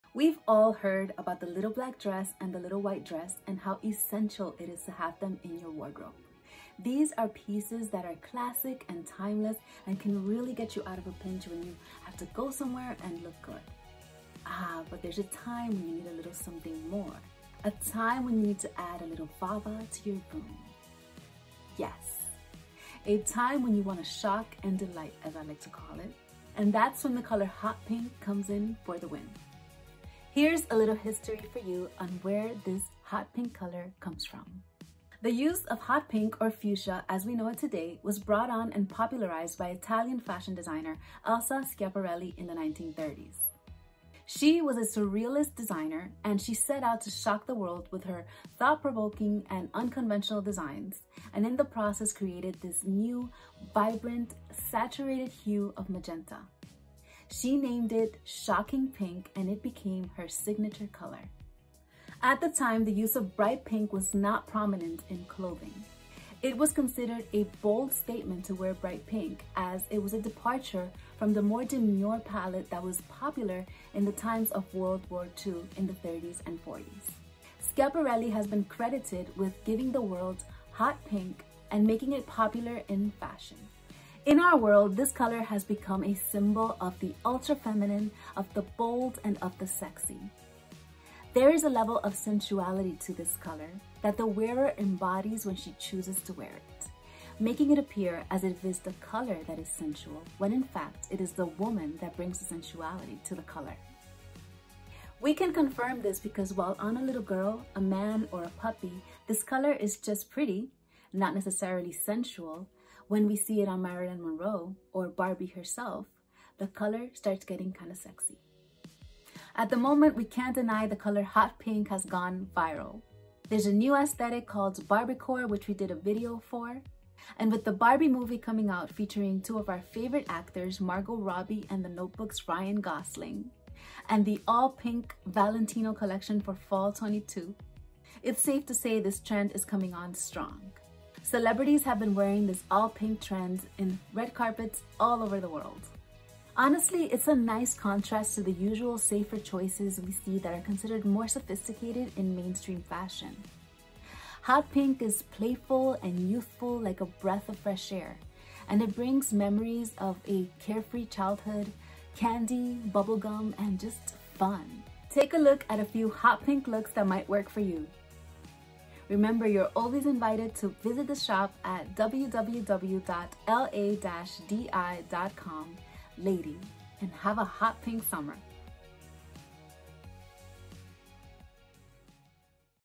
Barbiecore Trend Tips, The history of the Color Hot Pink by LaDi on YouTube Part 2
So we've all heard about the LBD - Little Black Dress and the Little White Dress- and how essential it is to have them as part of your wardrobe.  These are pieces that are classic and timeless, and can really get you out of a pinch when you have to go somewhere and look good.

Ahh, but there comes a time, when we want something more.  A time when we want to add a little va va to our voom.  Yes, a time when we want to shock and delight, as I like to think of it.  And that's where the color hot pink comes in for the win.

The Origin of Shocking Pink

The use of the color hot pink or fuchsia as we know it now was brought about and popularized in fashion by Italian designer Elsa Schiaparelli in the 1930's.  The surrealist designer set out to shock the world with her thought provoking unconventional designs and in the process introduced a saturated and vibrant new hue of magenta.  She named this new shade of pink "Shocking Pink" and it became her signature color.  At the time, the use of bright pink in clothing had not been prominent.  In the 30's and 40's wearing this color symbolized a bold statement as it was a departure from the more demure palette of the times which were plagued by World War II.  Schiaparelli is credited with giving the world what it's come to know as hot pink and popularizing it's use in fashion.

Hot Pink in Pop Culture

In our world this color has become a symbol of the ultra-feminine, of the bold and of the sexy.  There is a level of sensuality in this color that I believe the wearer embodies when she chooses to wear it, making it appear as if it is the color that's sensual, when in fact it's the woman that brings the sensuality to the color.

We can confirm this because on a little girl, on a man or on a puppy, this color is just pretty, not necessarily sensual.  While when you see it on Marilyn Monroe in Gentlemen prefer Blondes or on Barbie herself, the color starts getting kinda sexy.

Barbiecore is Trending

At the moment, the color hot pink has gone viral with the trending aesthetic called Barbiecore.  With the new Barbie movie being filmed starring two of our favorite actors Margot Robbie and 'The Notebook's' Ryan Gosling, and the all-pink collection presented by Valentino for Fall 2022 this past spring, this trend has come on strong.  Celebrities have been wearing the all-pink trend in red carpets all over the world.  It's a nice contrast to the usual safer choices considered to be more "sophisticated" in mainstream fashion.  It is playful and fun and youthful, like a breath of fresh air.  It brings back memories of a carefree childhood, candy, bubble gum and FUN!